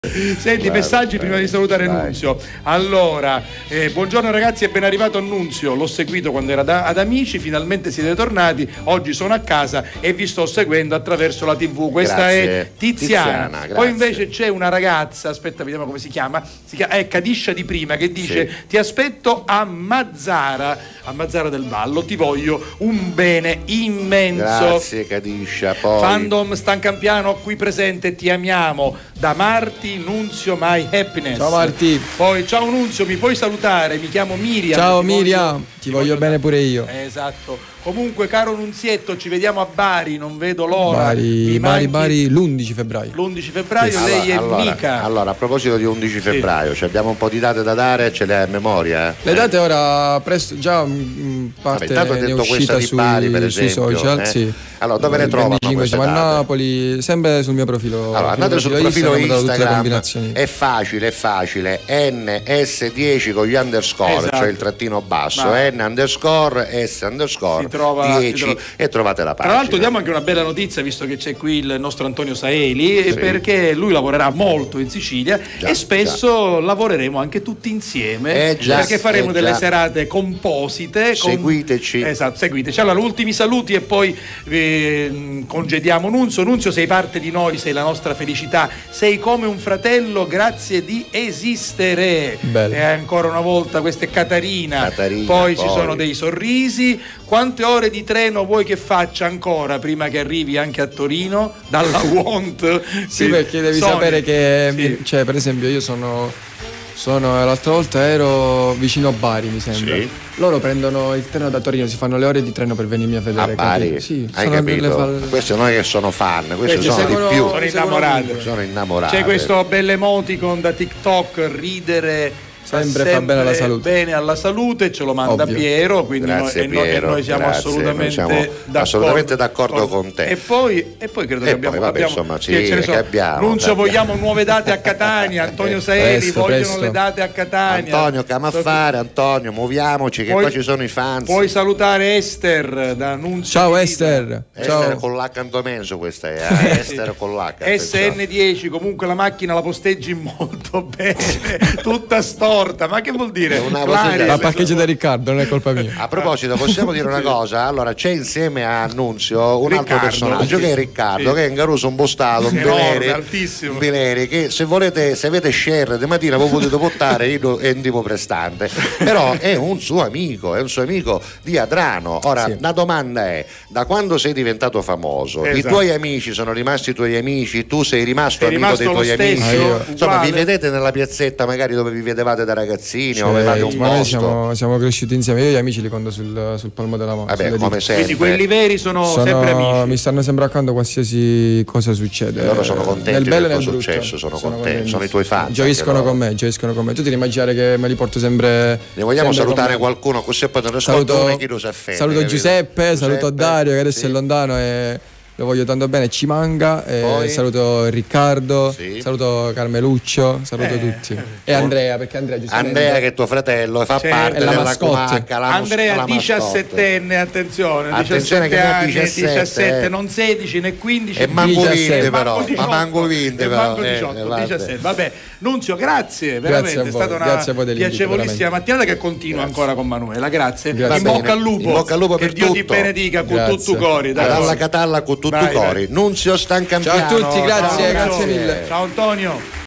Nunzio Stancampiano - Terza Parte
Ancora messaggi e qualche domanda al simpaticissimo Nunzio di Amici.